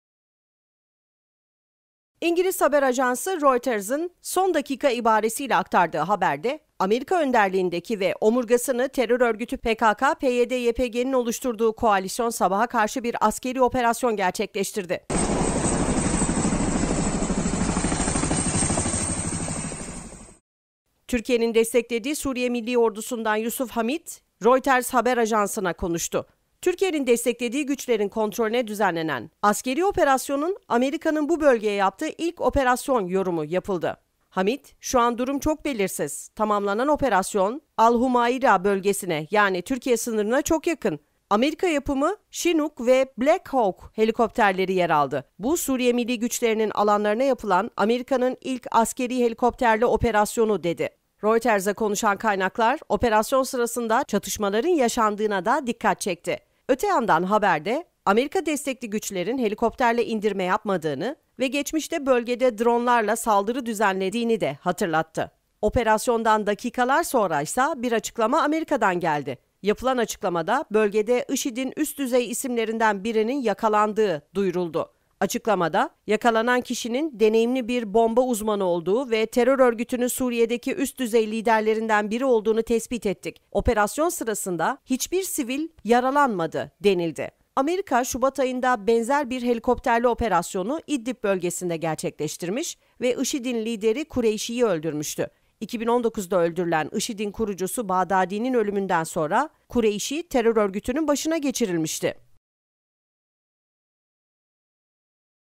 ABD Şafak Baskını Yaptı IŞİD Lideri Yakalandı
İngiliz haber ajansı Reuters’ın “son dakika” ibaresiyle aktardığı haberde ABD önderliğindeki ve omurgasını terör örgütü PKK/PYD/YPG’nin oluşturduğu koalisyon sabaha karşı bir askeri operasyon gerçekleştirdi.
Türkiye’nin desteklediği Suriye Milli Ordusu’ndan Yusuf Hamid, Reuters haber ajansına konuştu. Türkiye’nin desteklediği güçlerin kontrolüne düzenlenen askeri operasyonun “ABD’nin bu bölgeye yaptığı ilk operasyon” yorumu yapıldı.
Hamid, “Şu an durum çok belirsiz. Tamamlanan operasyon Al-Humaira bölgesine yani Türkiye sınırına çok yakın. ABD yapımı Chinook ve Black Hawk (Kara Şahin) helikopterleri yer aldı. Bu Suriye Milli Güçleri’nin alanlarına yapılan ABD’nin ilk askeri helikopterli operasyonu” dedi.
Reuters’a konuşan kaynaklar operasyon sırasında çatışmaların yaşandığına da dikkat çekti. Öte yandan haberde ABD destekli güçlerin helikopterle indirme yapmadığını ve geçmişte bölgede dronelar ile saldırı düzenlediğini de hatırlattı.
Operasyondan dakikalar sonra ise bir açıklama ABD’den geldi… Yapılan açıklamada bölgede IŞİD’in üst düzey isimlerinden birinin yakalandığını duyuruldu.
Açıklamada, “Yakalanan kişinin deneyimli bir bomba uzmanı olduğu ve terör örgütünün Suriye’deki üst düzey liderlerinden biri olduğunu tespit ettik. Operasyon sırasında hiçbir sivil yaralanmadı” denildi.
ABD, Şubat ayında benzer bir helikopterli operasyonu İdlib bölgesinde gerçekleştirmiş ve IŞİD’in lideri Kureyşi’yi öldürmüştü. 2019’da öldürülen IŞİD’in kurucusu Bağdadi’nin ölümünden sonra Kureyşi terör örgütünün başına getirilmişti.